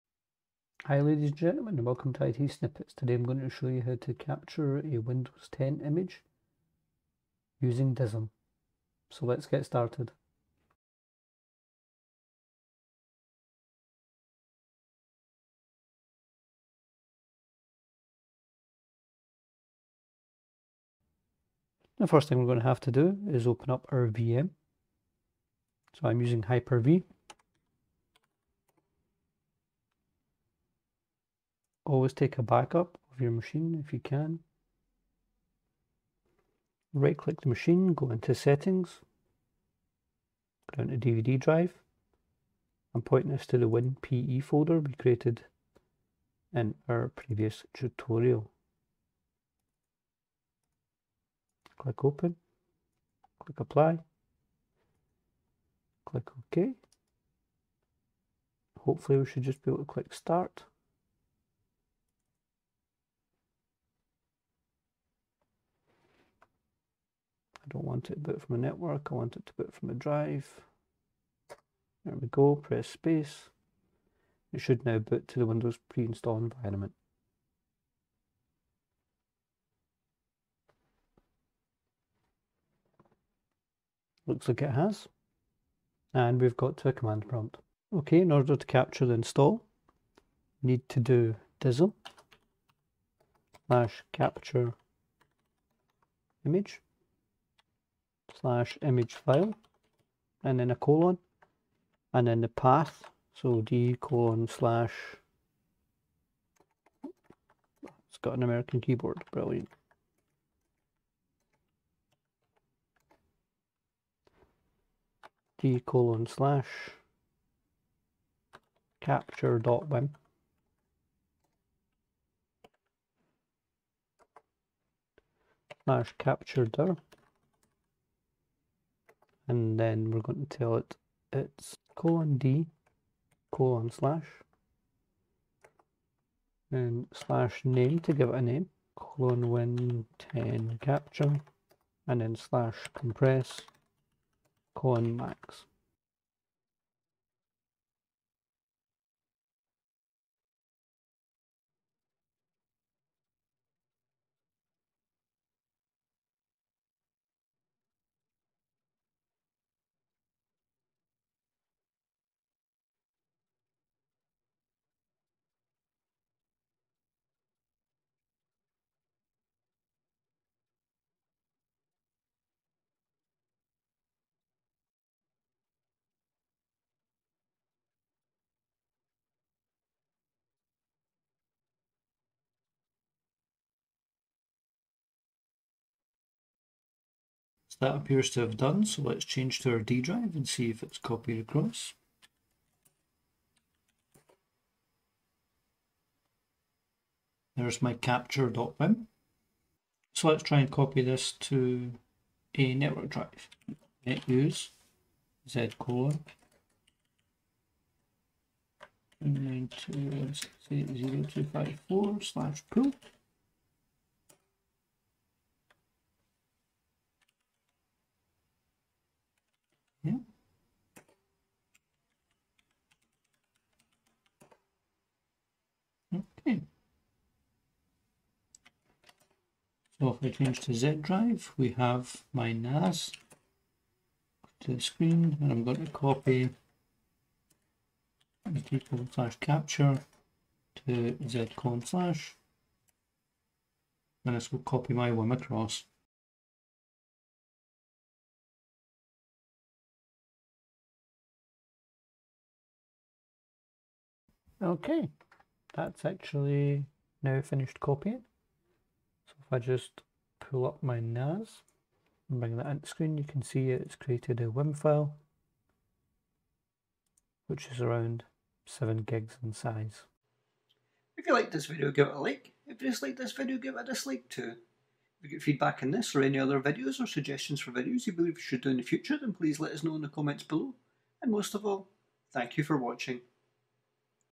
How To Capture A Custom Windows Image And Installed Apps Using DISM (@youtube
In this tutorial I show you how you can capture a custom windows install (I'm using Windows 10). This can be used to add to a Windows Deployment Server or to create a custom ISO (more custom than using slipstreaming) and I use windows PE and DISM.

*note* My mic freaks out in the middle.. no idea why!